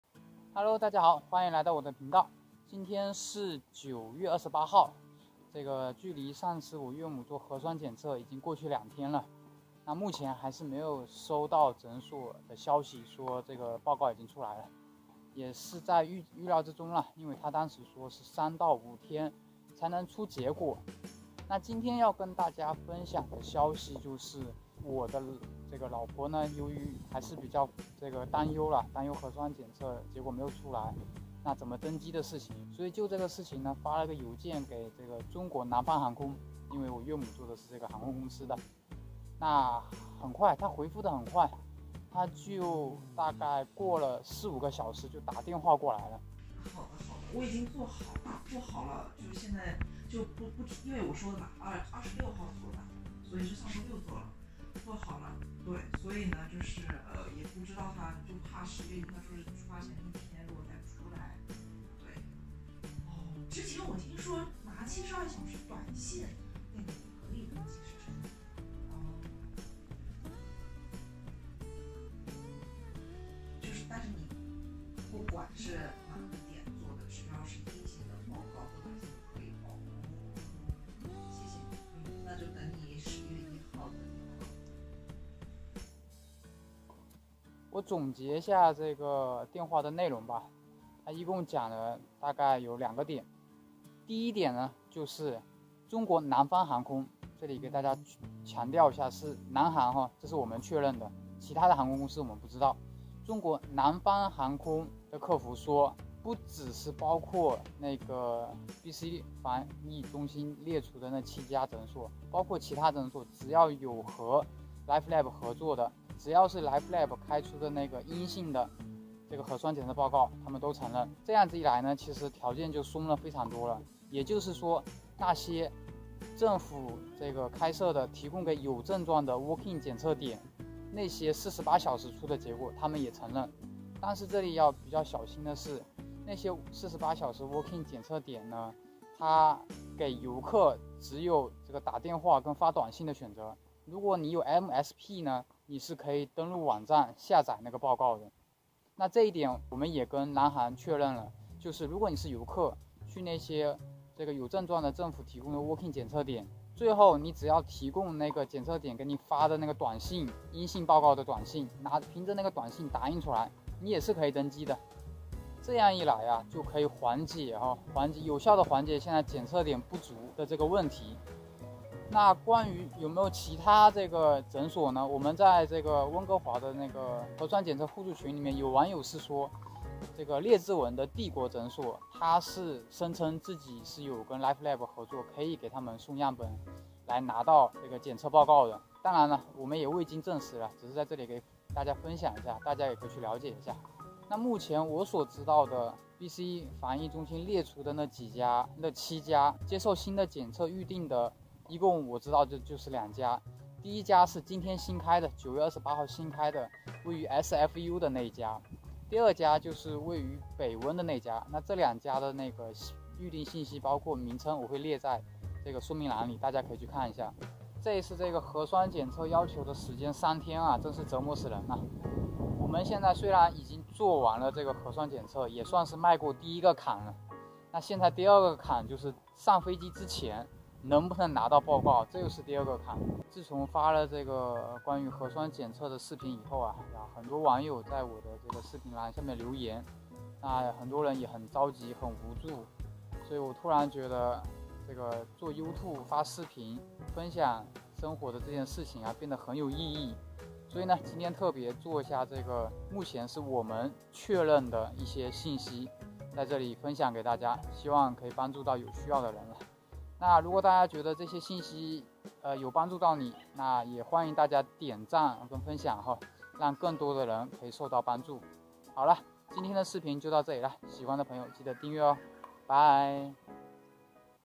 南航客服打电话来确认，关于温哥华核酸检测诊所选择的那些事|加拿大回国核酸检测（4）
南航客服刚和我们通完电话，有以下几个信息是我们和南航确认过的：检测核酸的诊所不限，只要是lifelab出具阴性结果就可以。所有那种给有症状的walk in 48小时出结果的测试点也可以，如果是游客没有msp，去walk in测试点最后只能发短信通知你测试结果。南航确认过，凭阴性短信结果也可以登机。（但是去walk in点测试的都是有症状的，还是有感染风险的，不建议去）
另一种，就是去找和lifelab合作的可以送检测样本的诊所咯，有网友说列治文的帝国诊所自称可以测回国人员的核酸检测（未证实）

目前BC防疫中心列出的7家诊所中还接受短期内新的预约的诊所：
北温的检测点：Integrated Wellness Medical Centre (Tri-Cities)